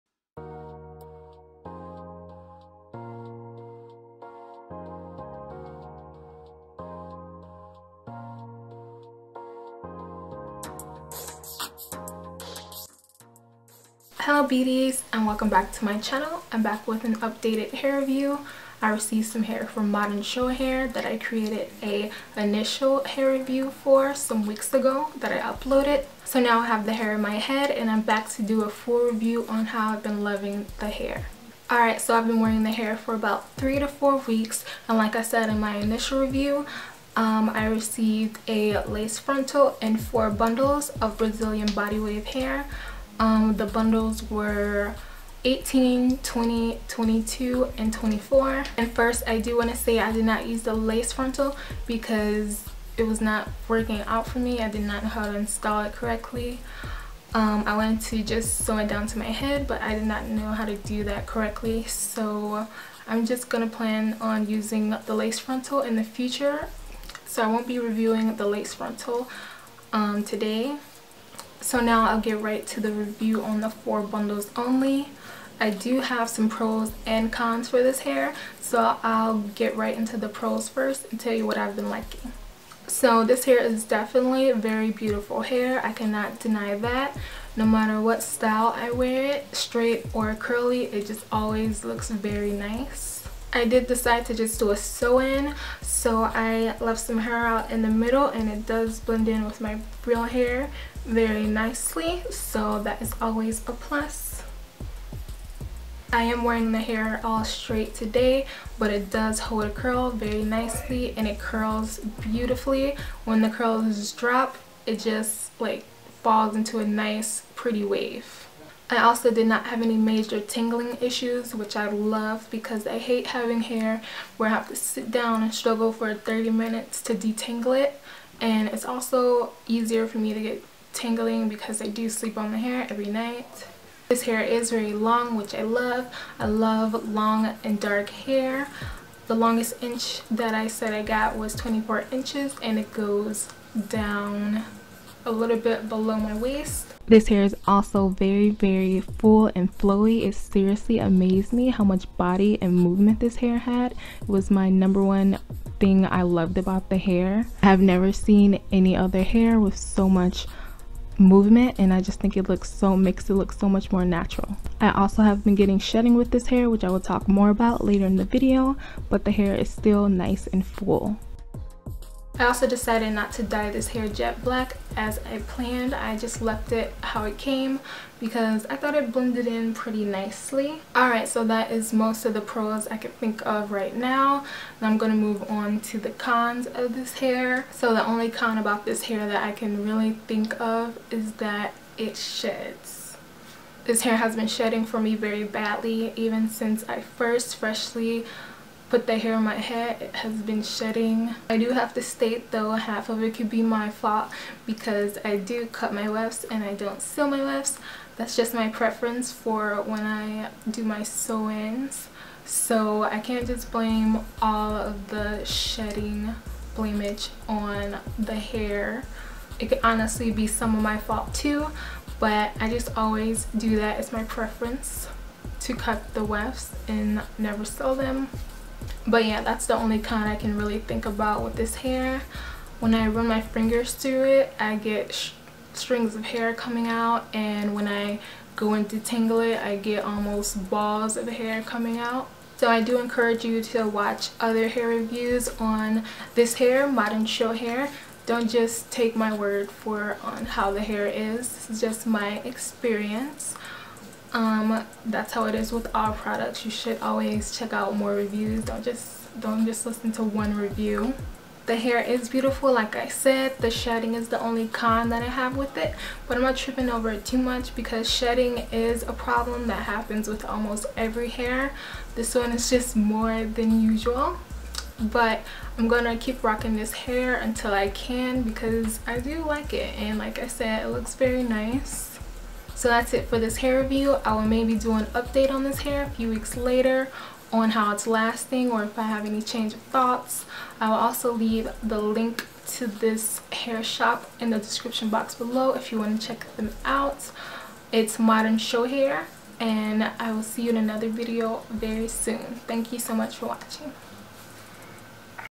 BOMB ALIEXPRESS HAIR CHEAP! MODERN SHOW HAIR REVIEW | BRAZILIAN BODY WAVE
Back with a full updated review on the Modern Show Hair!

Modern Show Hair Direct Store

3 Bundles + Frontal

Four Bundles

Four Bundles + Frontal

Three Bundles

3 Bundles + Closure